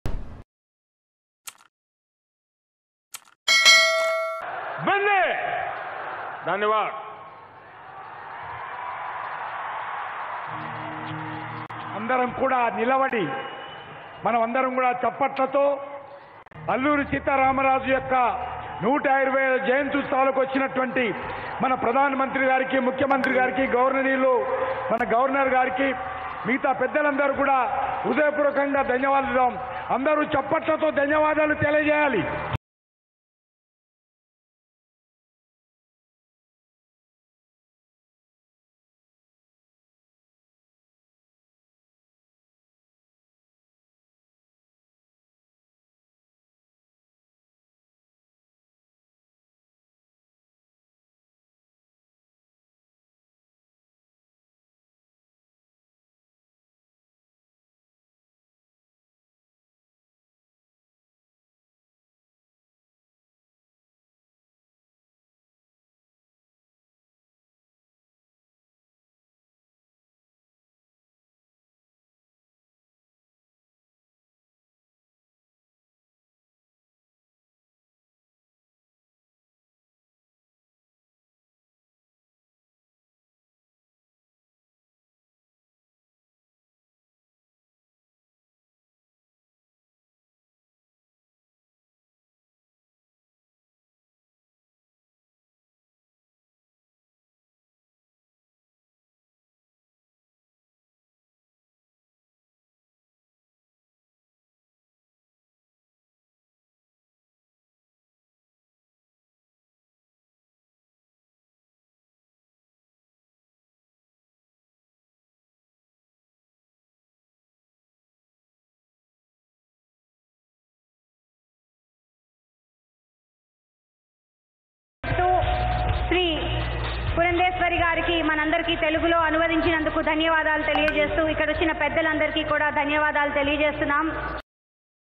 చిరంజీవికి ఆత్మీయ పలకరింపు... సెల్ఫీ కోసం మంత్రి రోజా పాట్లు... ప్రధని సభలో చిత్రాలు... | PM Modi
చిరంజీవికి ఆత్మీయ పలకరింపు... సెల్ఫీ కోసం మంత్రి రోజా పాట్లు... ప్రధని సభలో చిత్రాలు...